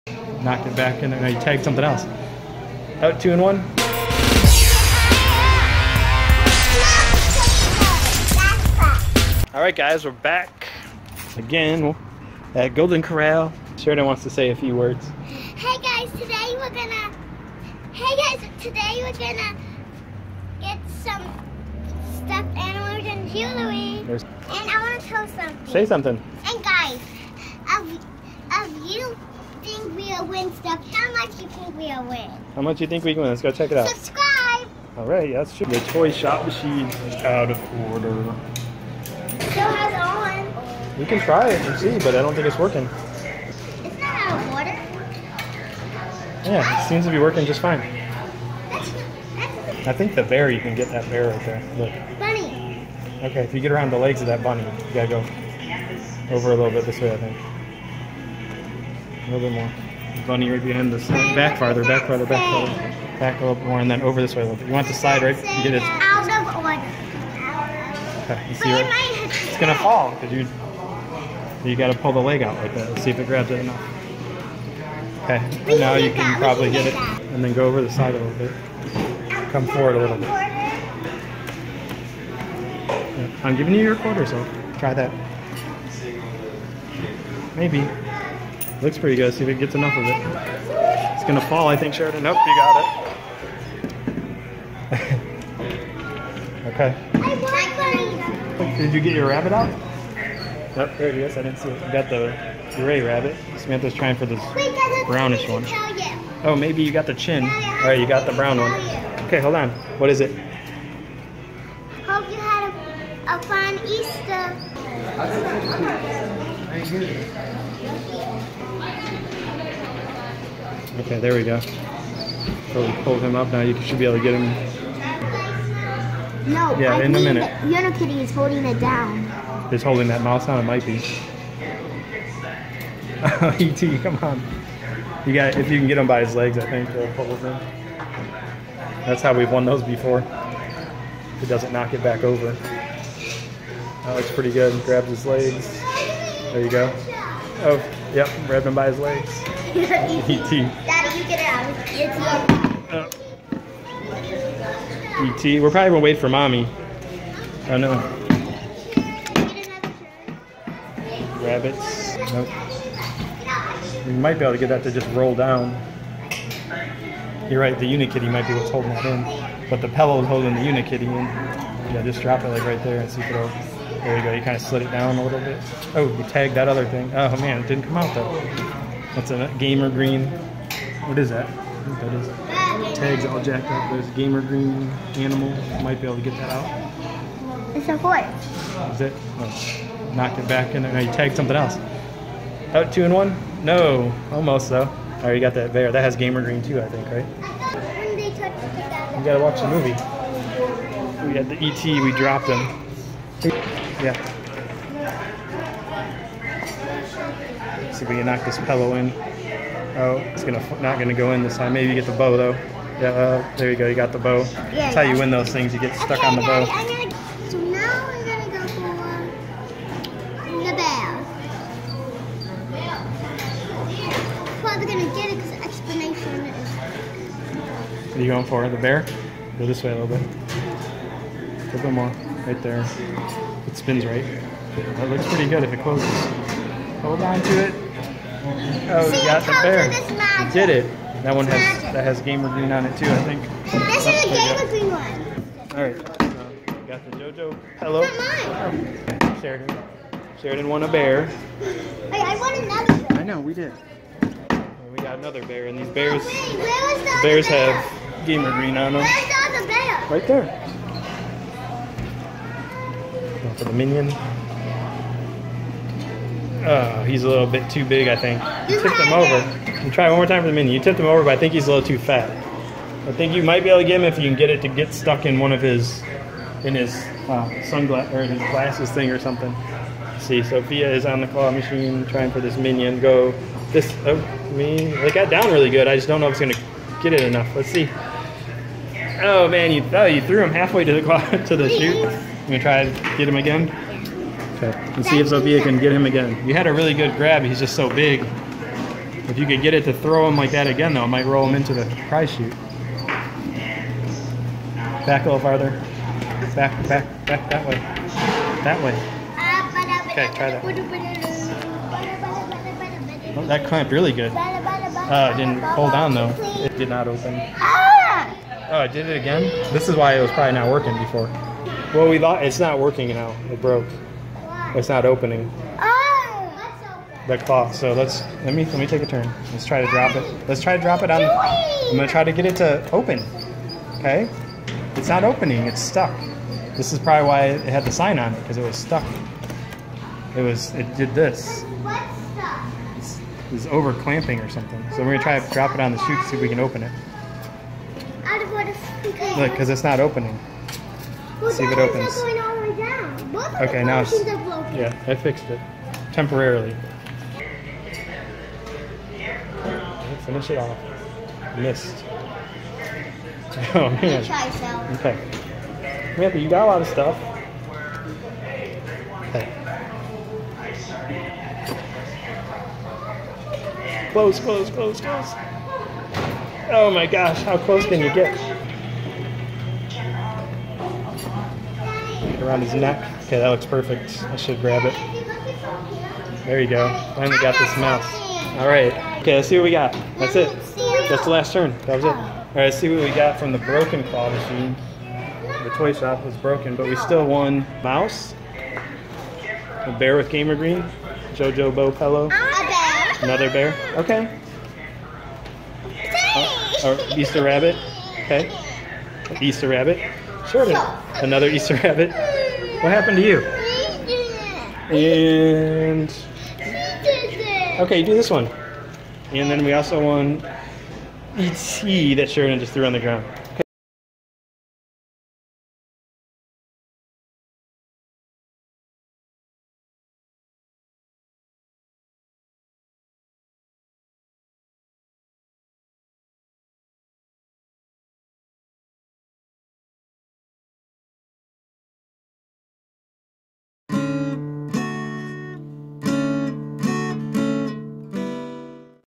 Broken Claw Machine (Can We Win Any Prizes?) Out Of Order GamerGreen Arcade Game Room Crane
Broken claw machine, can we win any prizes?  We played the out of order GamerGreen Arcade game room crane machine.  Can we win any prizes?  If you even see a broken claw machine, give it a try, sometimes it still works.  We still won a ton of prizes from the out of order claw machine.  We even won some GamerGreen from the game room prize crane.